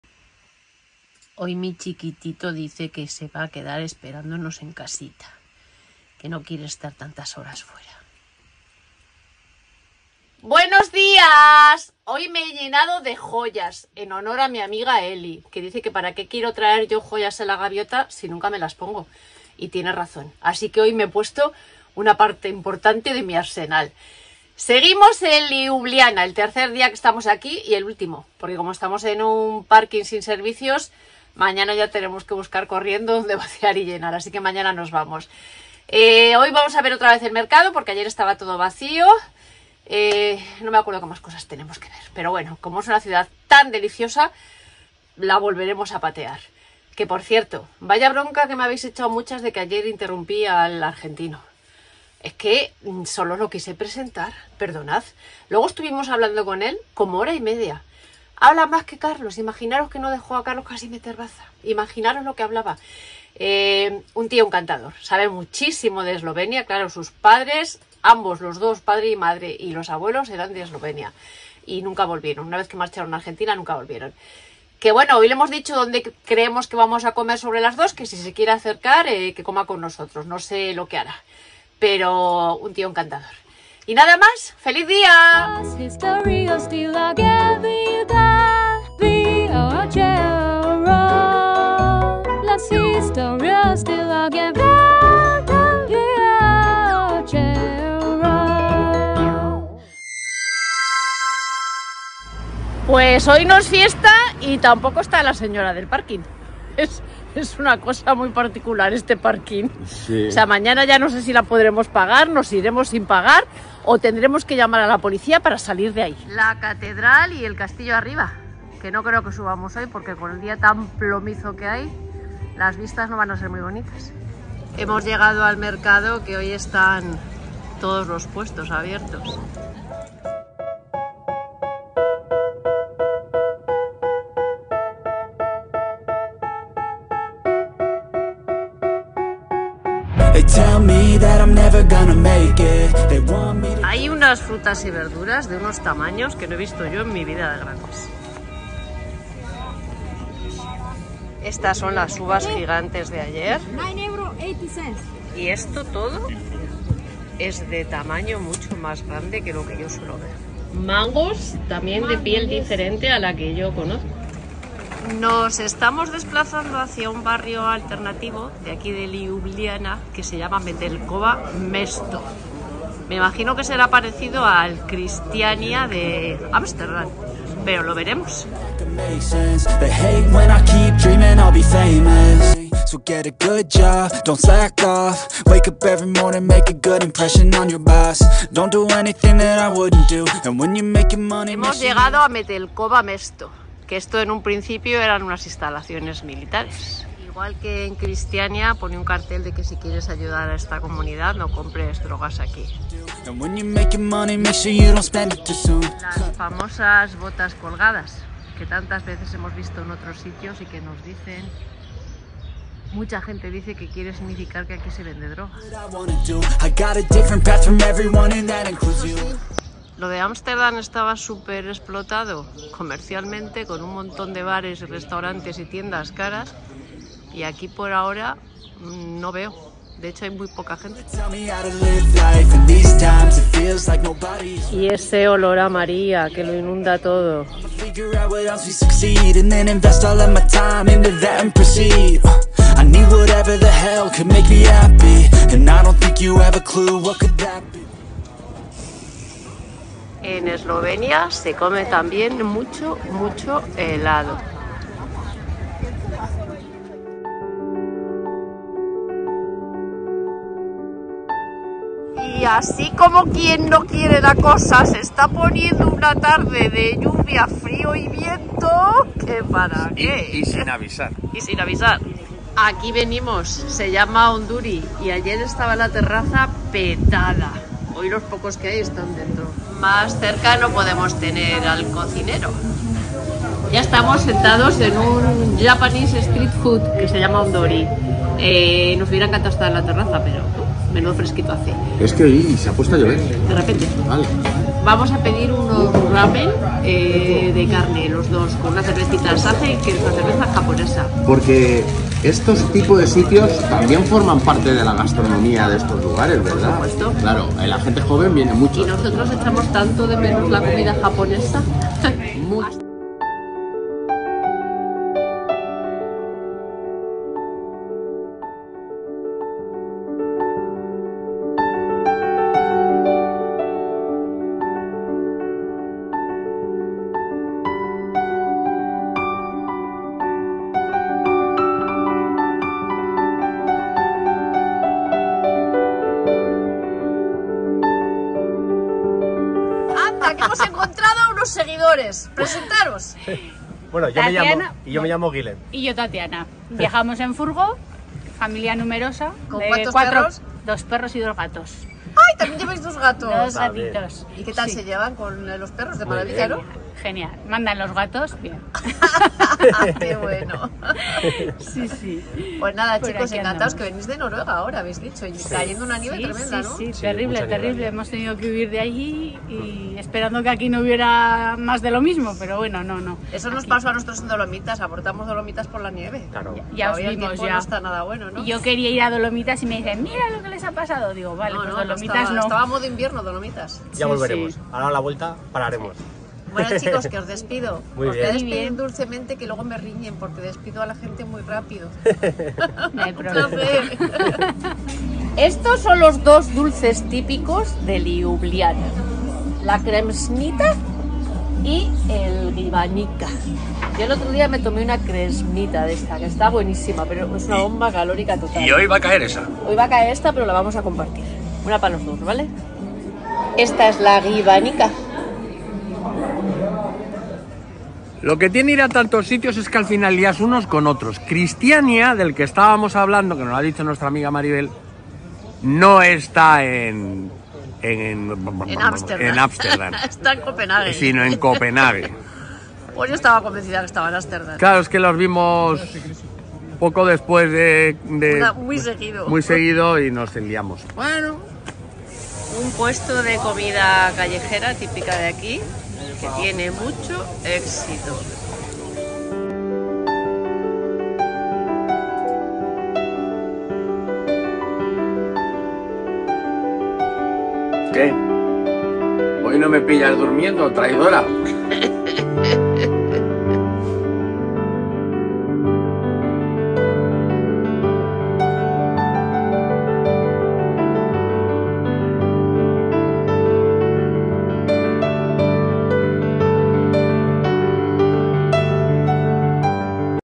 Qué BRONCA me HABÉIS ECHADO Abril 2023 # 623
Tercer y último día en Liubliana (Eslovenia).